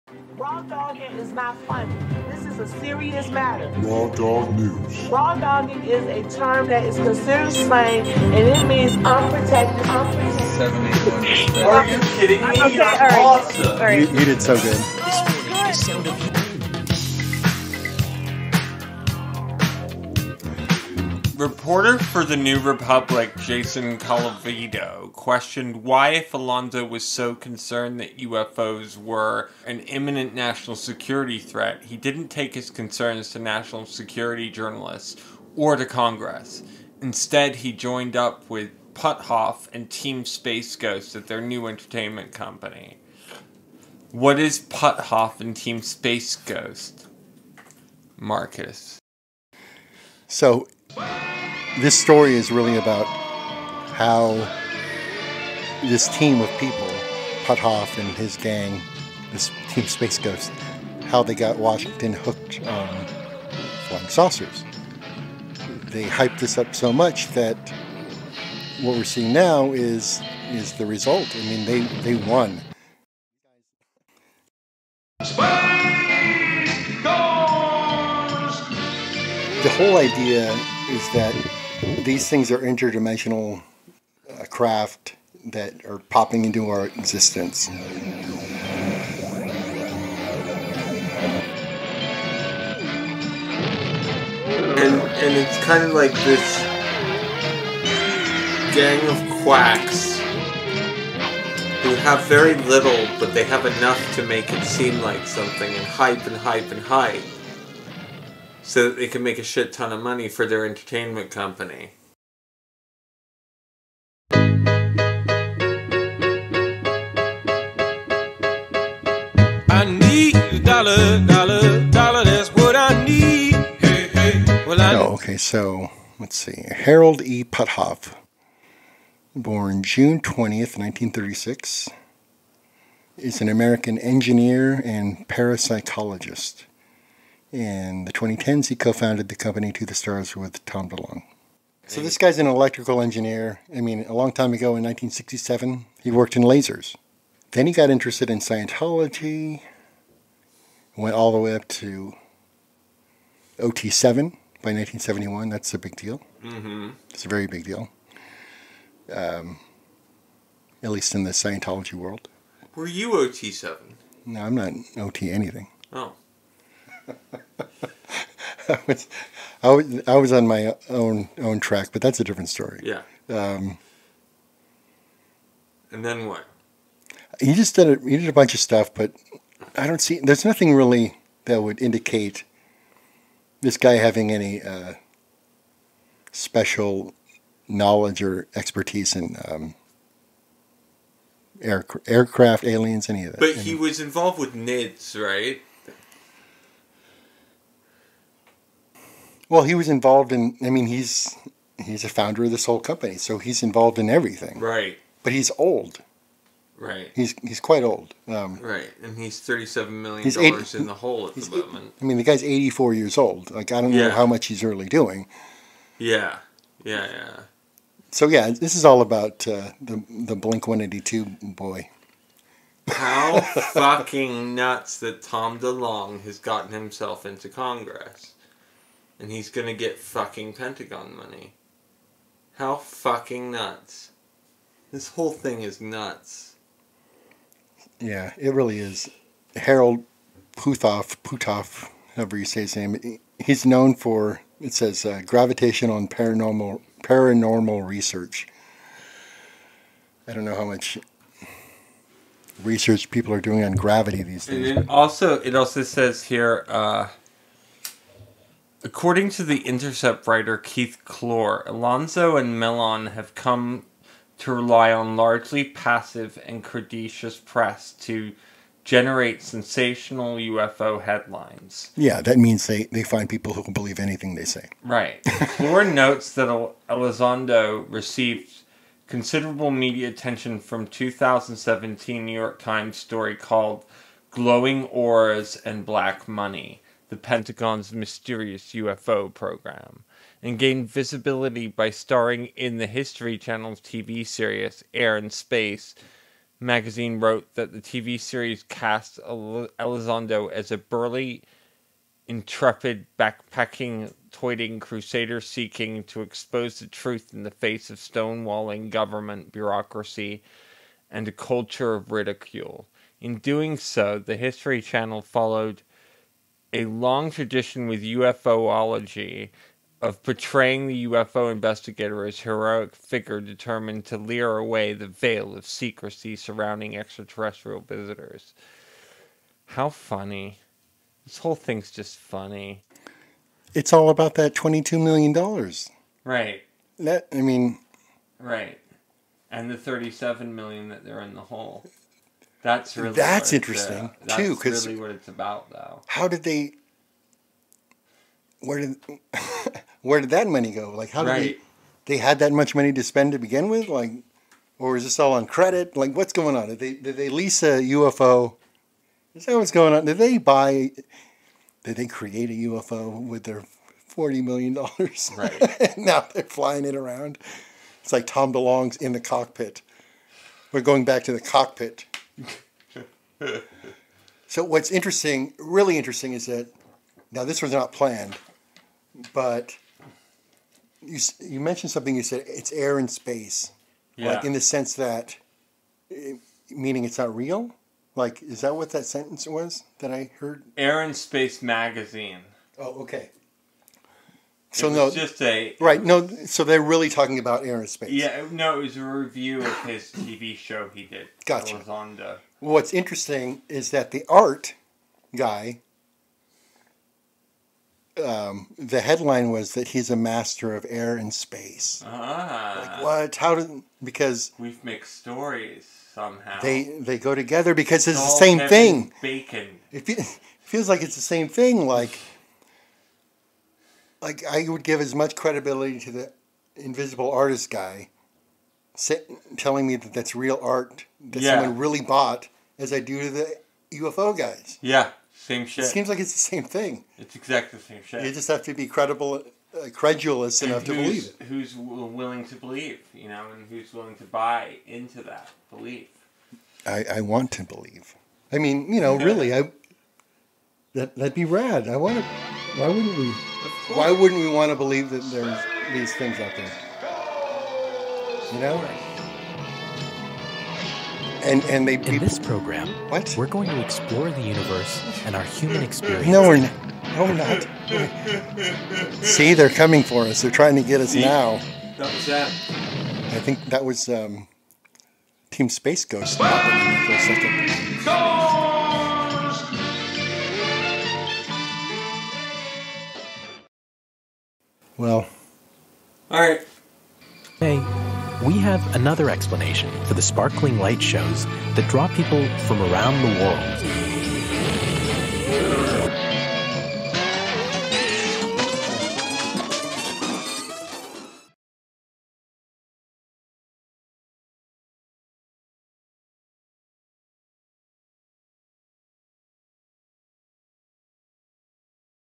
Spaced Force: The Blink-182 Connection To The UAP Sightings
Alexei and Marcus take a look at the current UFOs UAPs USOs phenomenon and what it's all about.

Raw Dog Twitter @thedogofraw

Call that Raw Dog Hotline any time and leave a hot take or bad opinion and we may play it on the show.